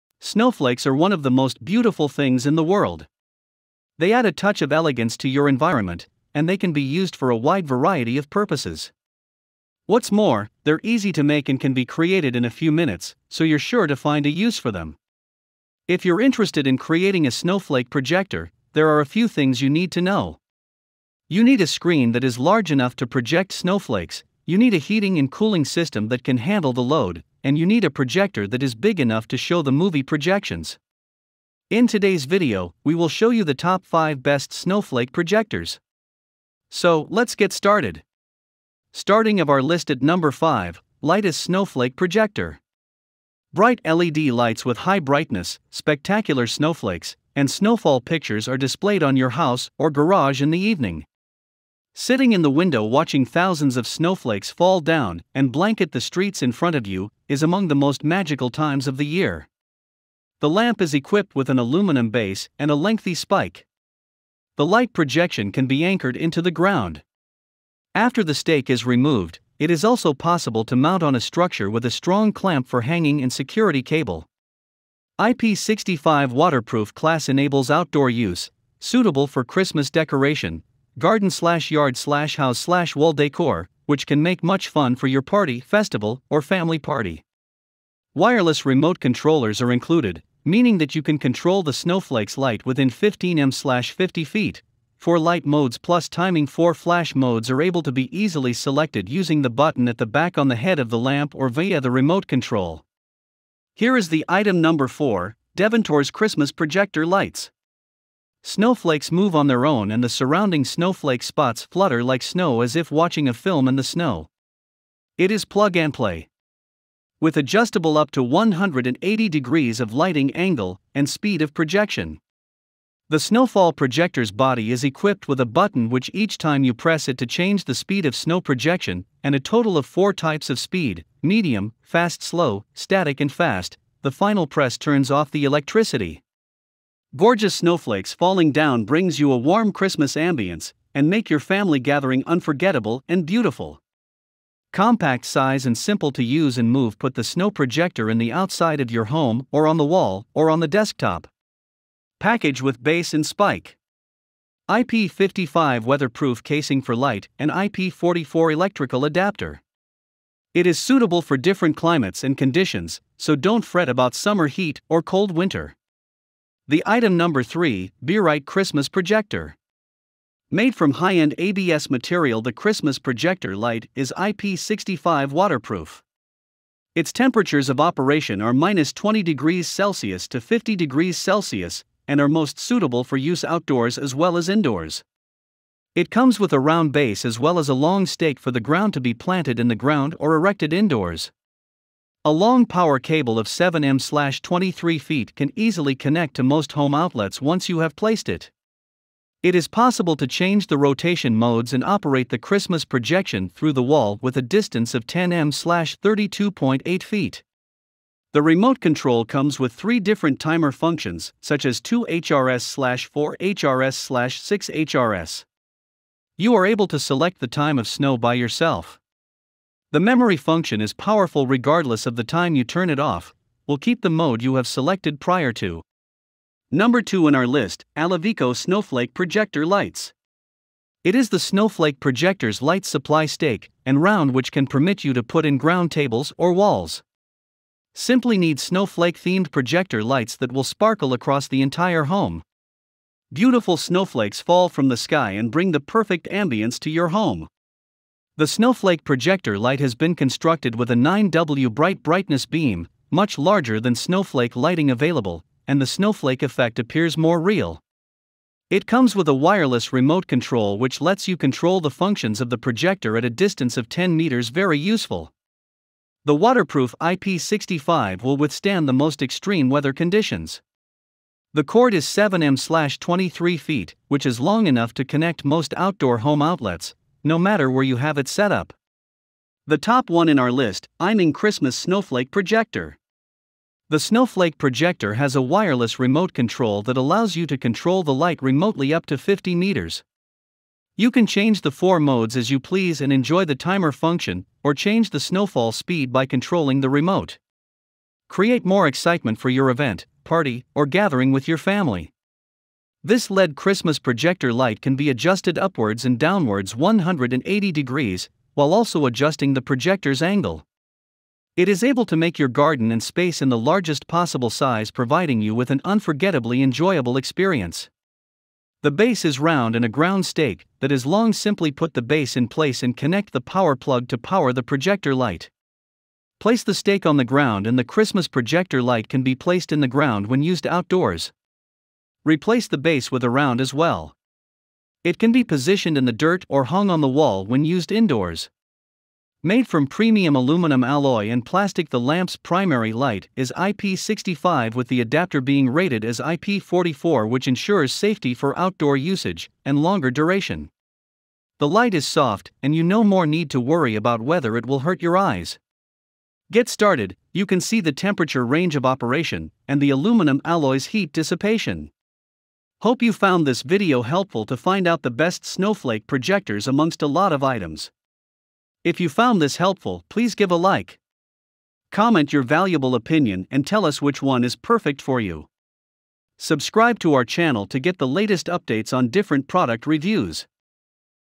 Top 5 Best Snowflake Projector
5. Lightess Snowflake Projector

4. DEVENTORZ Christmas Projector Lights

3. B-right Christmas Projector

2. ALOVECO Snowflake Projector Lights

1. YMING Christmas Snowflake Projector